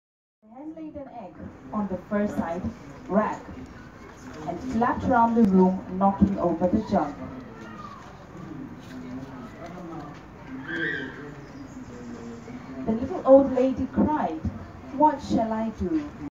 Grace Msalame reads
Grace Msalame reads a story to children at the World Storytelling Day hosted the Kwani grounds in Nairobi on 19th March 2016.